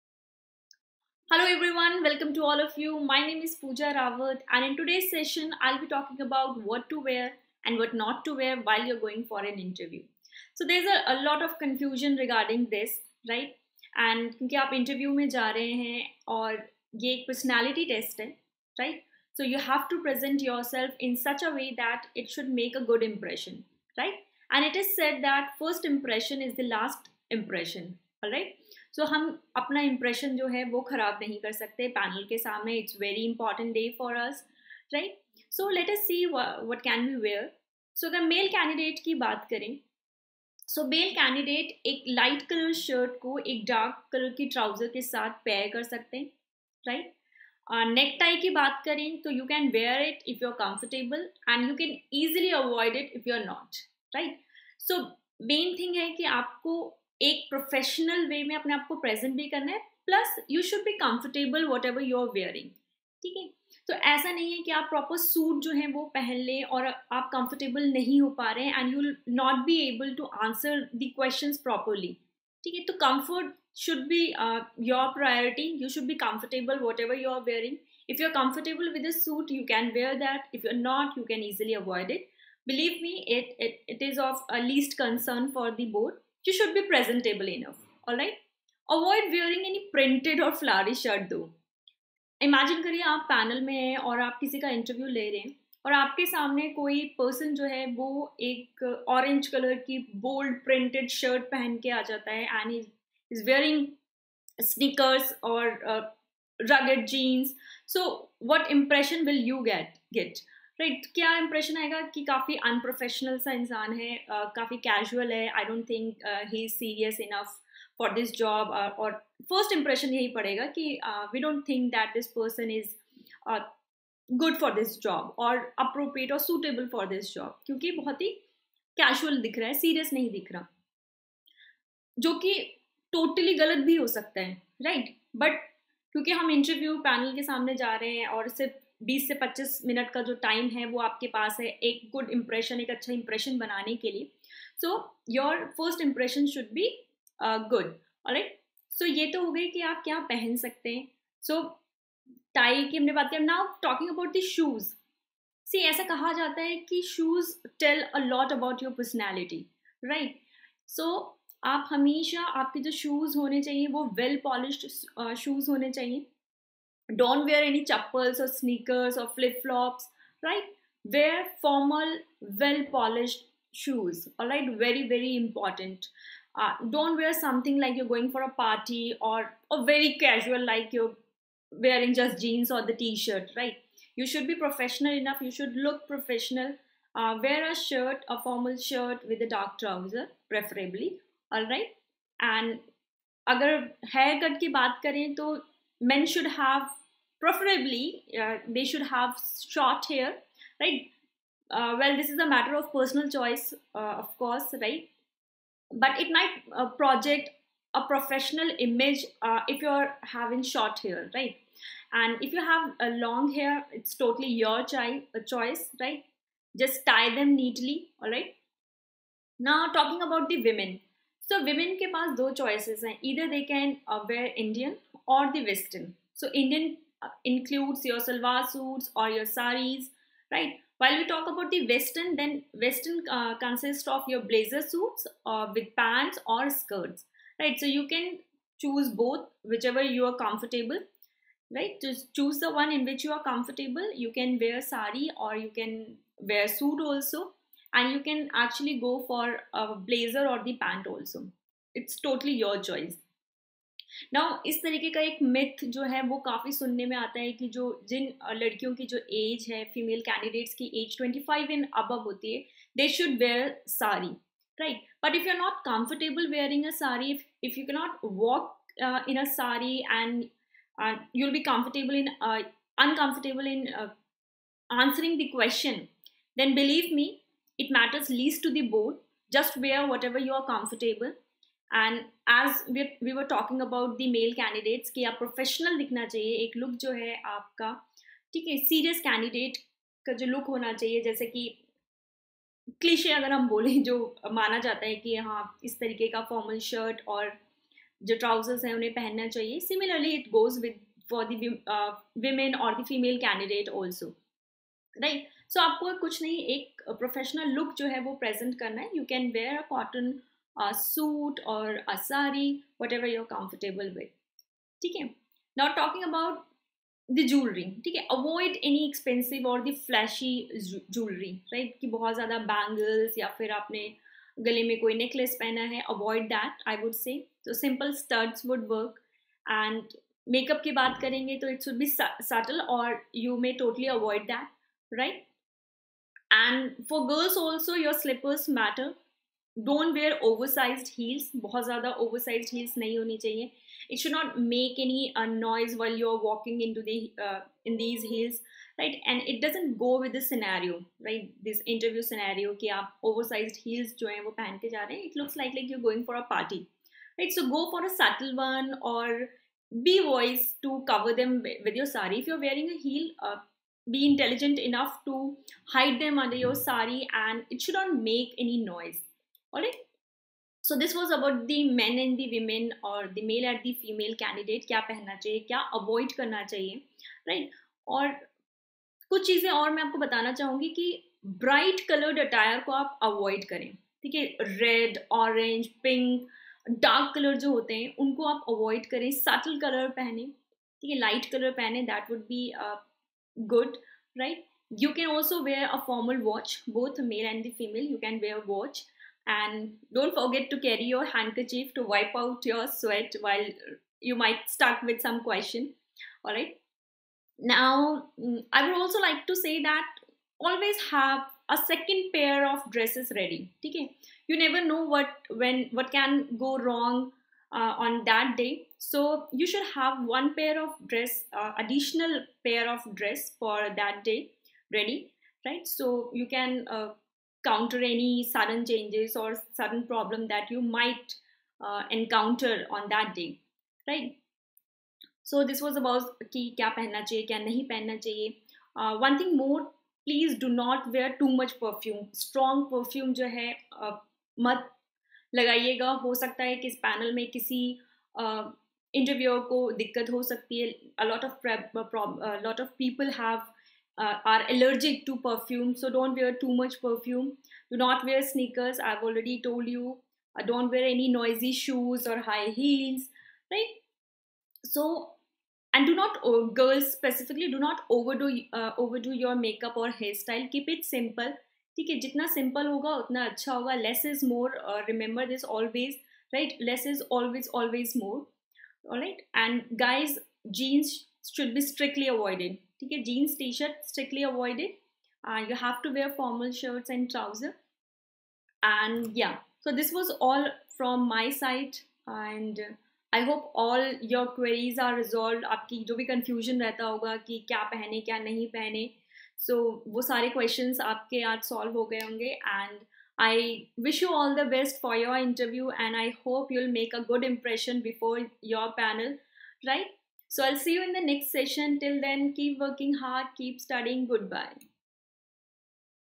What to wear for an interview| NABARD Grade A by - Pooja Ma'am
In today's session I'll be resolving all your queries regarding the Interview dress code. We will be discussing what should be worn while going for an interview and what should be avoided.